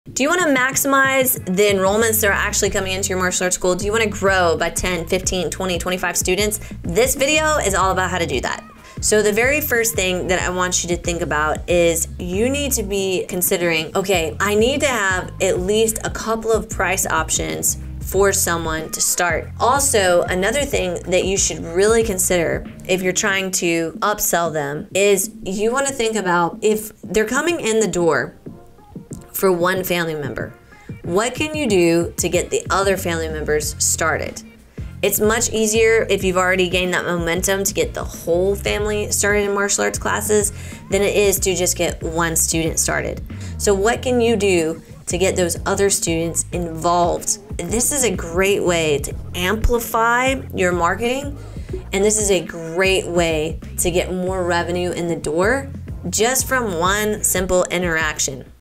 Maximizing Your Martial Arts School's Enrollments Through Upselling and Cross Selling Techniques
Jennifer Waters is a professional fighter, CEO, wife, and mom to Beckett. She is a life long martial artist and competitor as well as a business savvy female entrepreneur with one of her businesses doing 7+ figures a year.